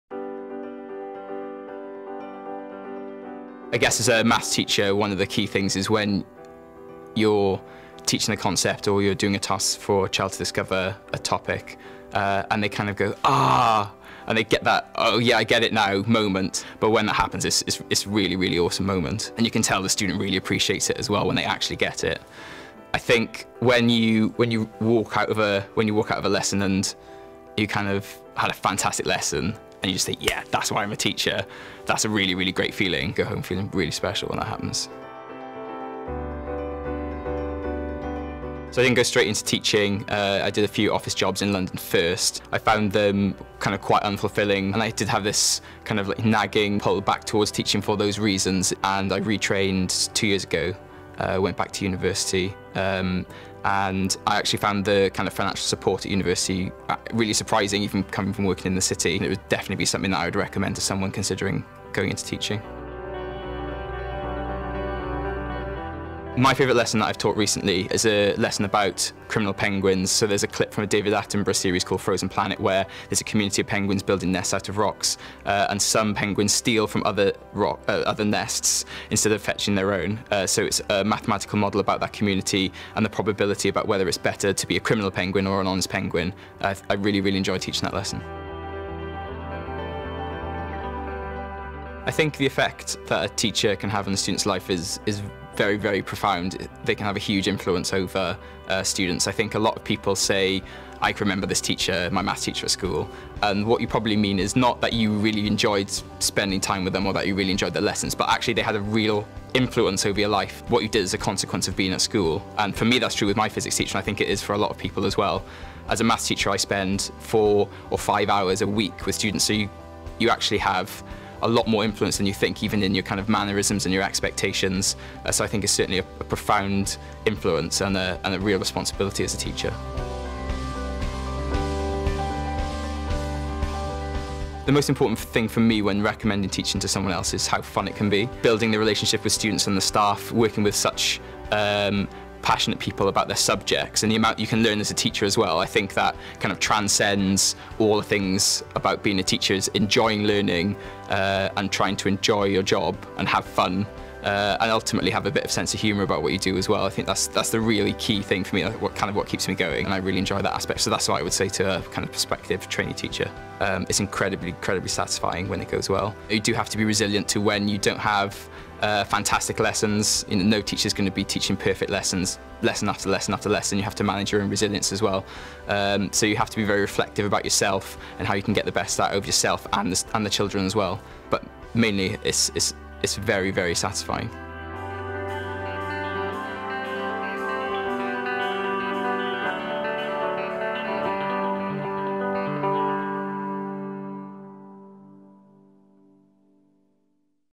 Mr Ben Davies, maths teacher
Watch maths teacher Ben Davies talking about teaching his favorite lesson about ‘criminal penguins’,  and the difference a maths teacher can make on a child’s life.